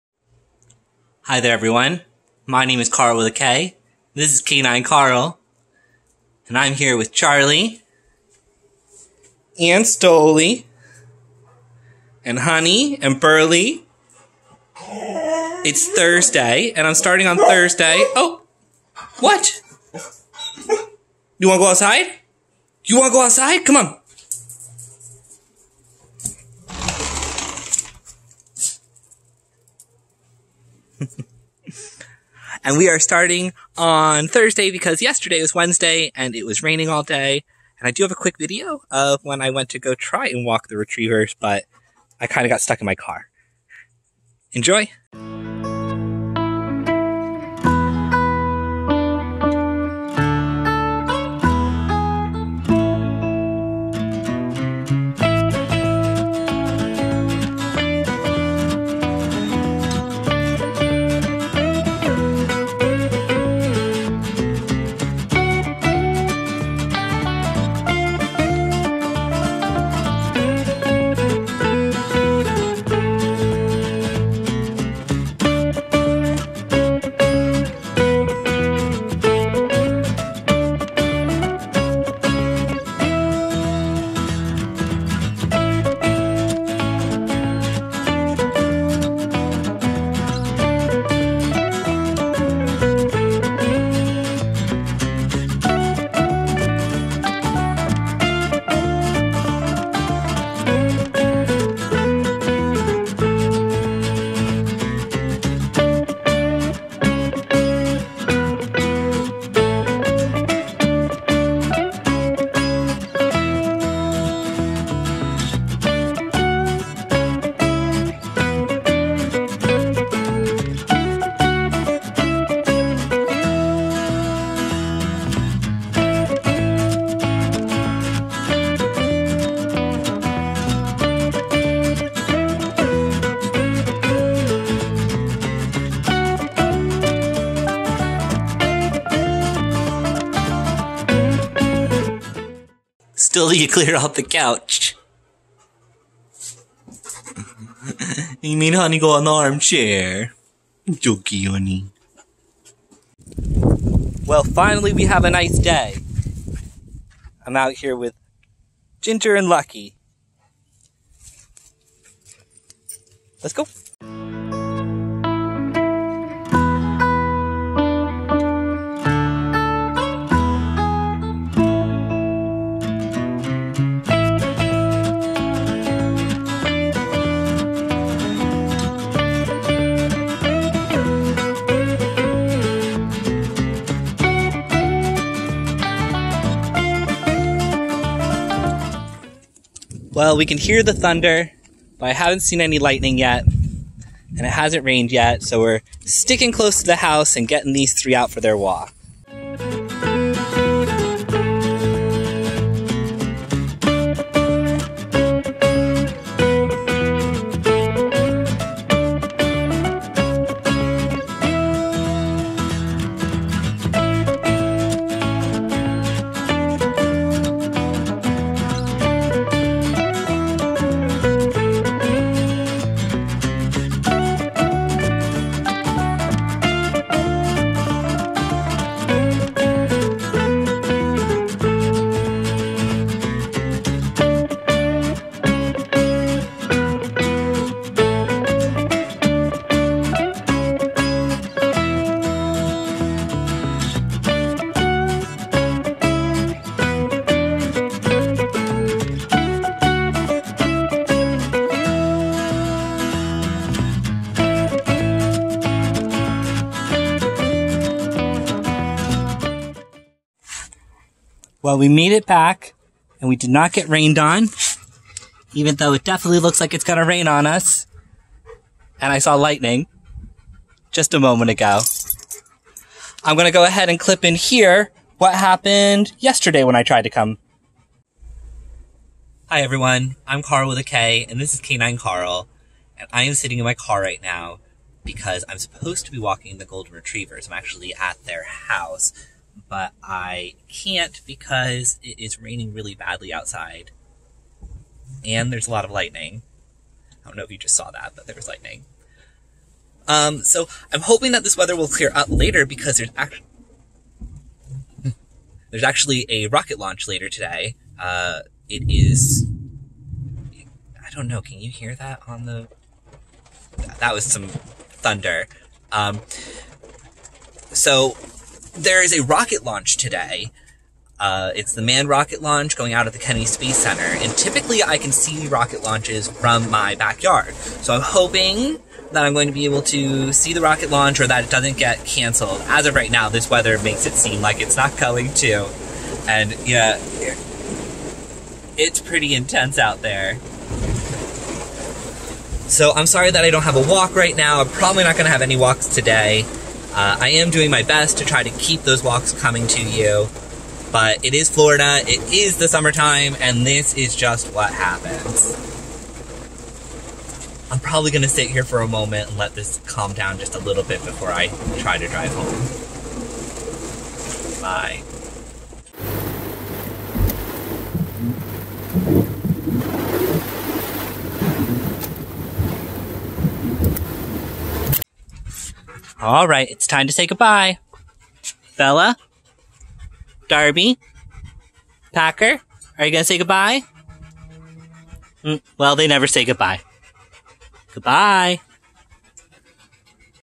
Golden Retrievers Walk, Get Rained Out Wednesday
It was a very rainy Wednesday, followed by a very busy Thursday.